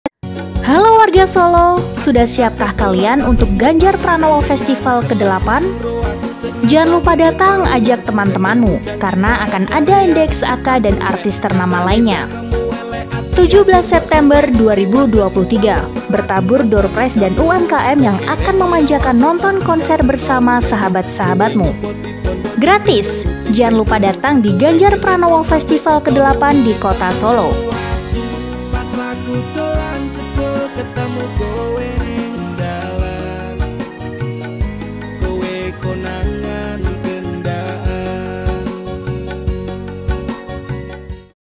KOTA SOLO SIAP BERGEMURUH DENGAN GELARAN GANJAR PRANOWO FESTIVAL #8
Bersenang-senanglah dalam Ganjar Pranowo Festival yang diadakan di Solo, Jawa Tengah pada minggu, 17 September 2023.

Kamu juga berkesempatan untuk membawa pulang doorprize yang sangat menggiurkan, karena GPF ini diadakan secara Gratis. Tidak lupa juga akan ada UMKM yang ikut memeriahkan pesta rakyat ini.

Acara ini juga akan dimeriahkan dengan penampilan dari Jogja Hiphop Foundation, NDX AKA, Souljah, Abah Lala, NDARBOY Genk dan Stevan Pasaribu. Bersiaplah untuk bersenang-senang dalam GPF #8 Solo ini!!